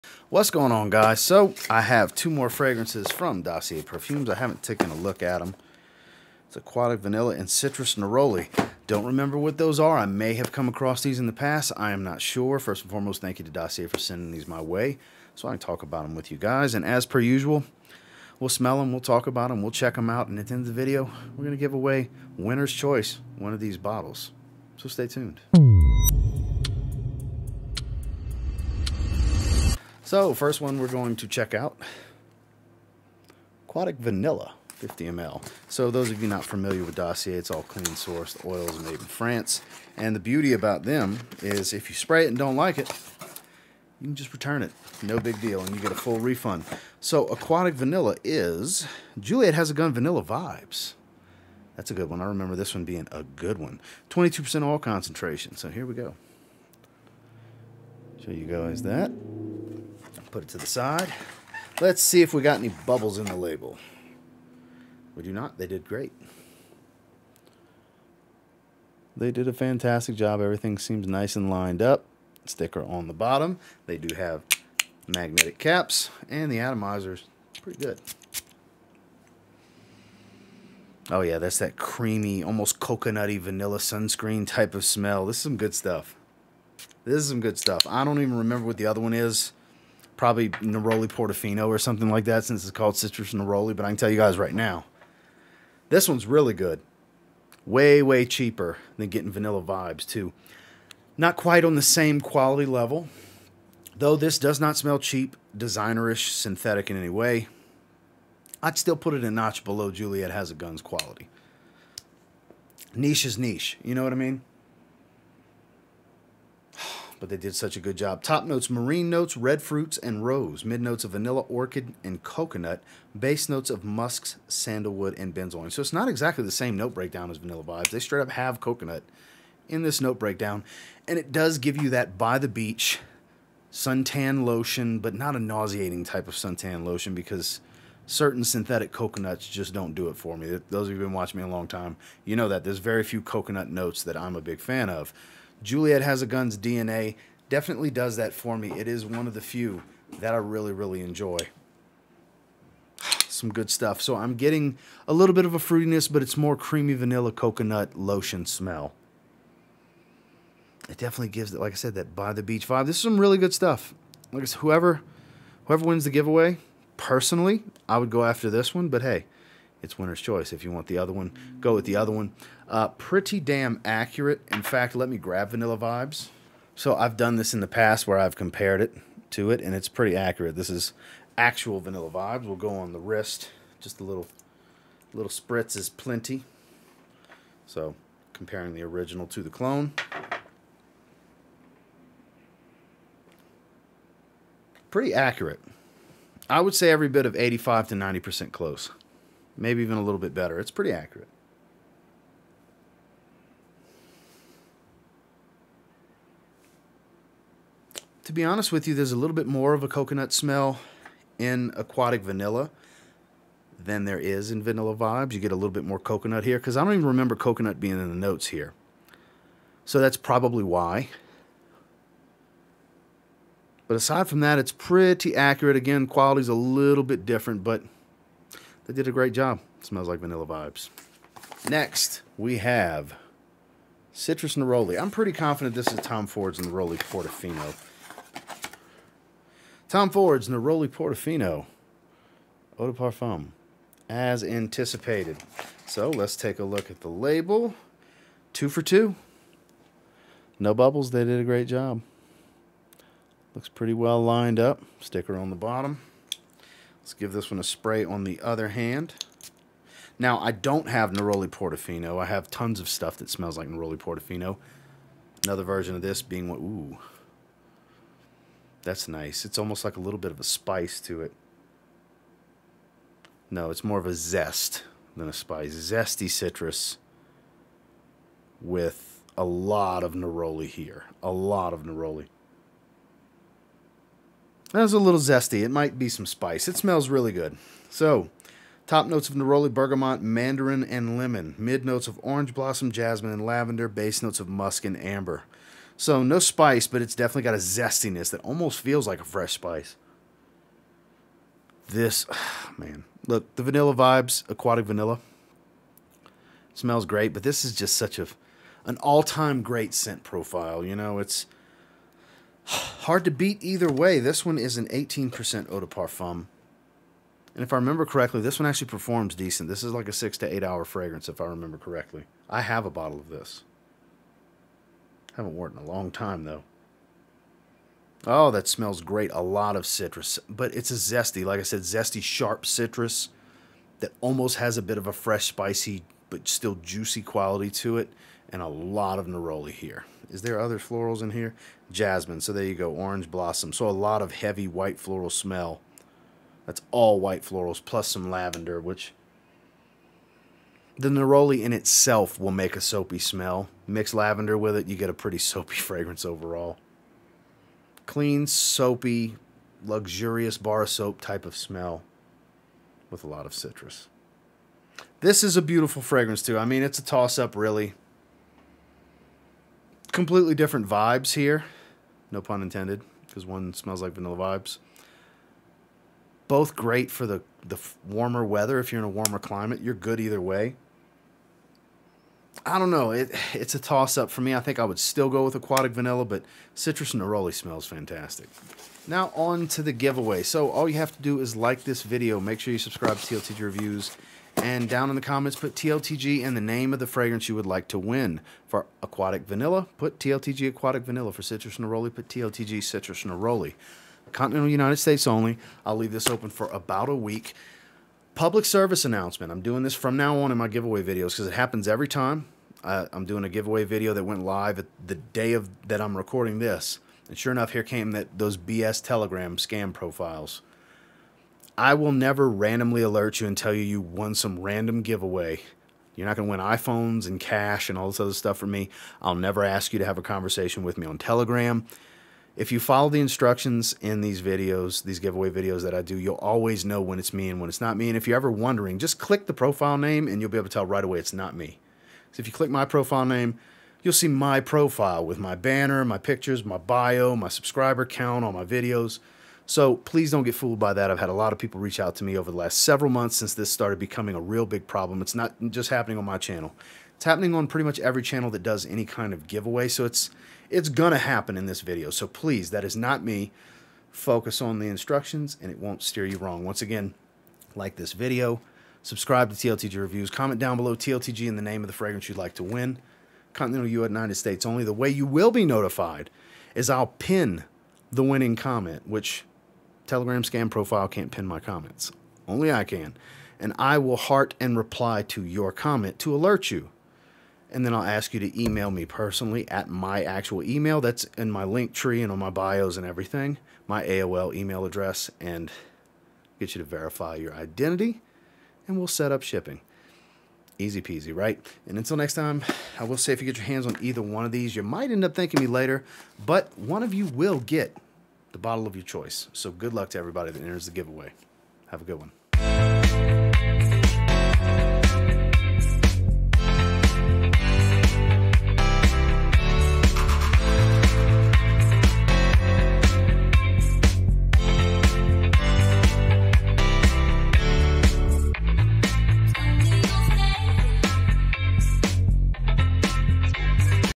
Hot Weather Bangers on a Budget? | Dossier Perfumes (CLOSED Giveaway)
Fragrances Featured:

Aquatic Vanilla "Juliette Has A Gun Vanilla Vibes"
Citrus Neroli "Tom Ford Neroli Portofino"

*The extra 10% applies at the checkout on top of the welcome offer*